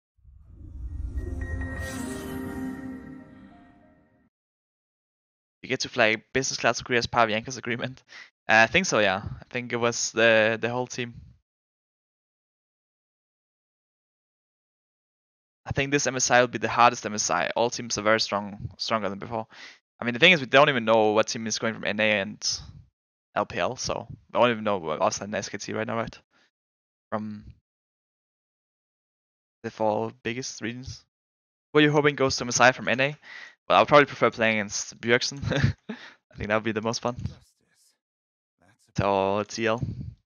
G2 Caps About MSI 2022 ✈
I Want To Play With BJEGSEN !
This Is A Hardest MSI All Team Are Very Strong !